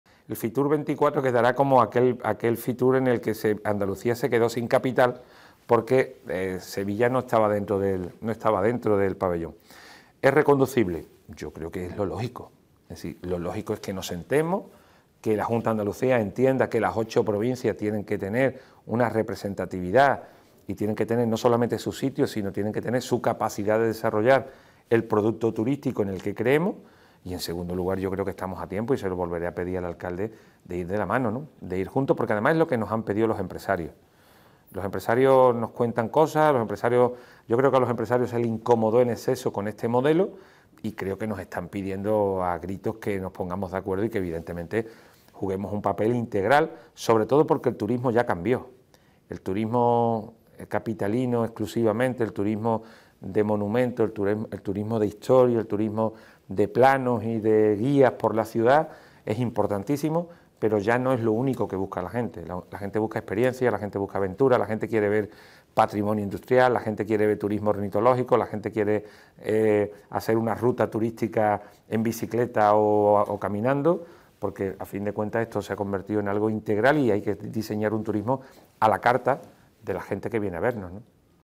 Fernández sobre Fitur: "Lo lógico sería reconducir el desencuentro e ir de la mano con Sevilla"
El presidente de la Diputación de Sevilla, Javier Fernández, considera que tras la última experiencia en la Feria Internacional de Turismo de Madrid (Fitur), donde la provincia y la capital acudieron por separado, "lo lógico sería reconducir el desencuentro e ir de la mano" a la próxima edición de este importante escaparate del sector.

En una entrevista en Europa Press, el mandatario provincial apuesta por que las distintas administraciones se sienten a dialogar y "que la Junta de Andalucía entienda que las ocho provincias tienen que tener una representatividad; no solamente su sitio, sino su capacidad de desarrollar el producto turístico en el que creemos".